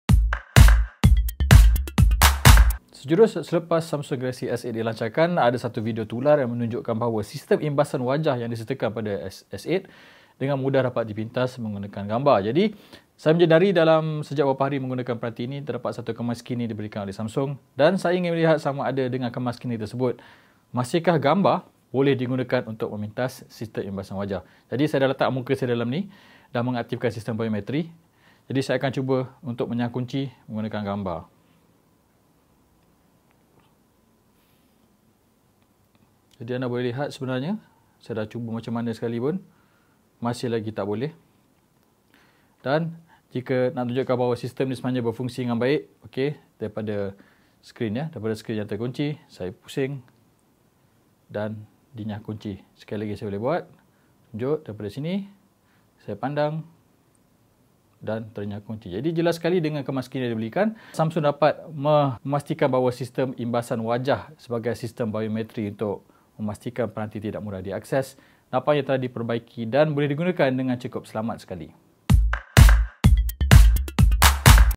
Bolehkah Sistem Imbasan Wajah Samsung Galaxy S8 Dipintas Selepas Dikemaskini?
Adakah sistem imbasan wajah Samsung Galaxy S8 masih mudah dipintas selepas dikemaskini? Ini video yang akan menjawab persoalan sekuriti anda.